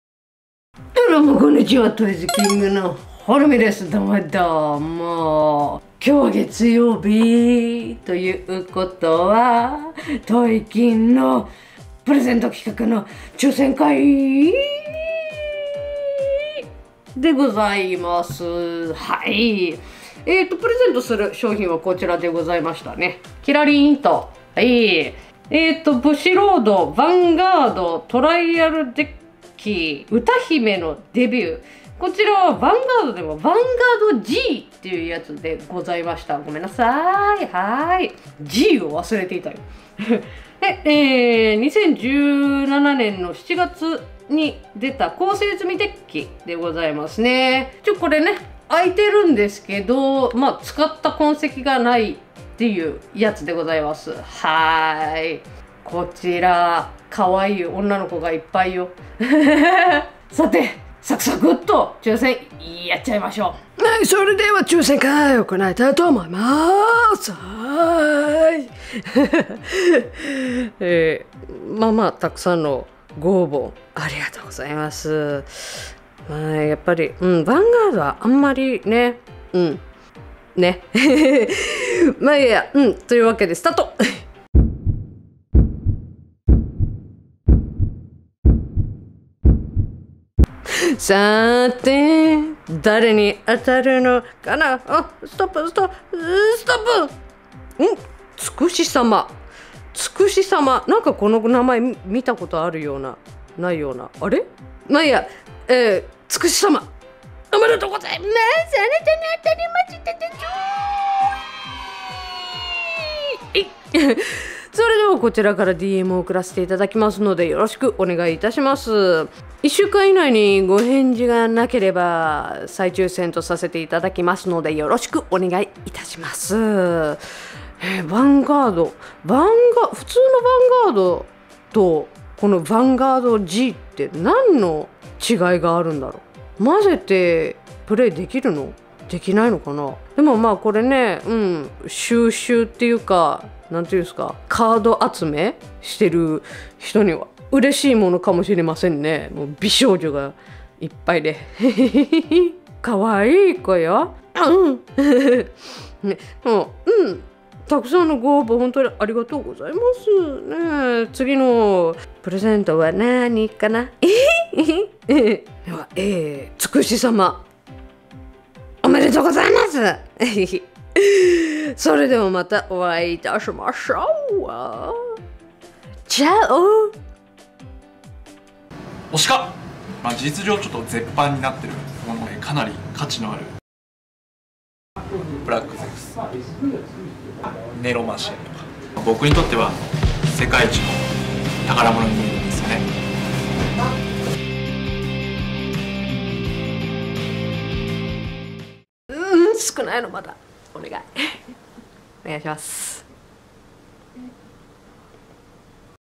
Twitterプレゼント企画動画の当選者発表！～【ヴァンガードG∼歌姫のデビュー」1BOX開封分】の抽選会～開封済みでも使った痕跡がない！そして美少女カードが…！！！
Twitterプレゼント企画動画の 今後もプレゼント企画いろいろ発信していく予定です！